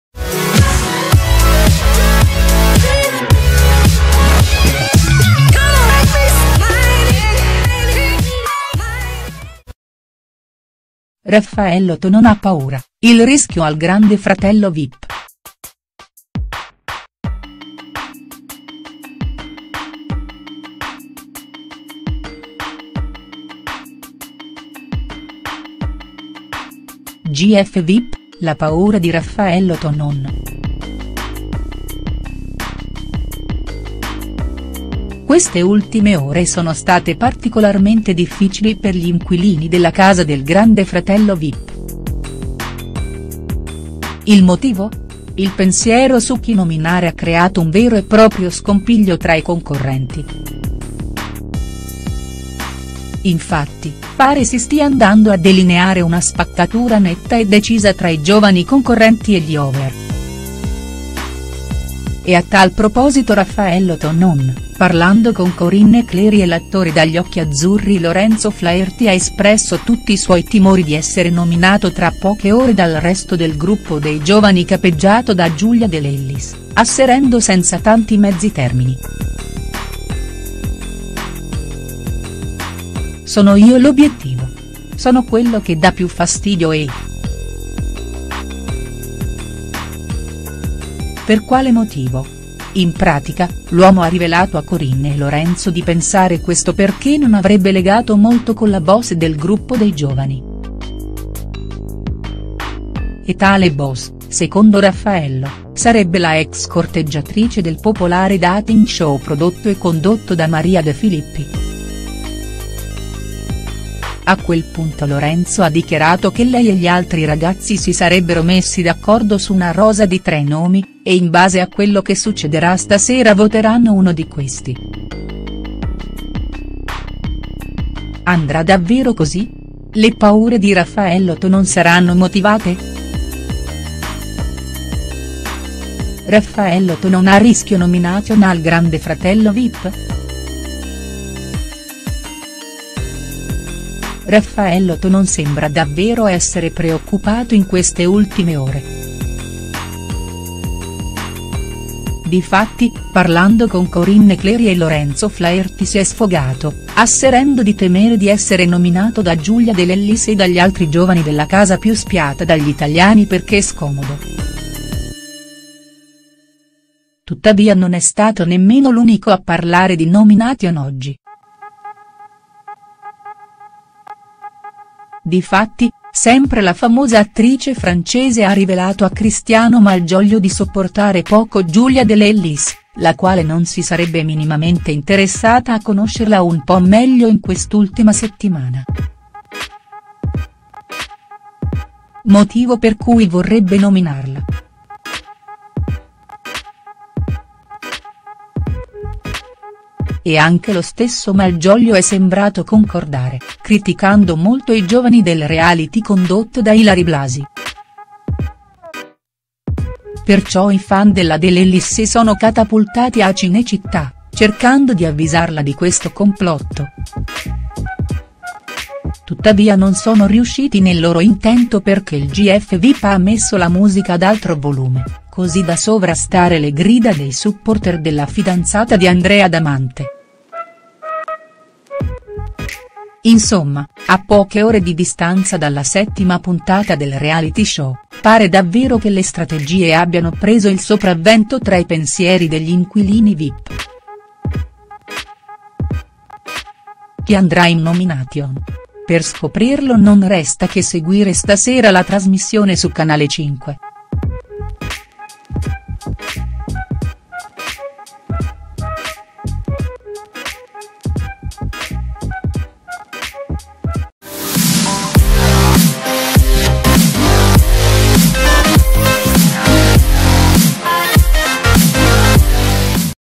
Raffaello Tonon ha paura: il rischio al Grande Fratello Vip| STARS NEWS
GF Vip: la paura di Raffaello Tonon Queste ultime ore sono state particolarmente difficili per gli inquilini della casa del Grande Fratello Vip. Il motivo?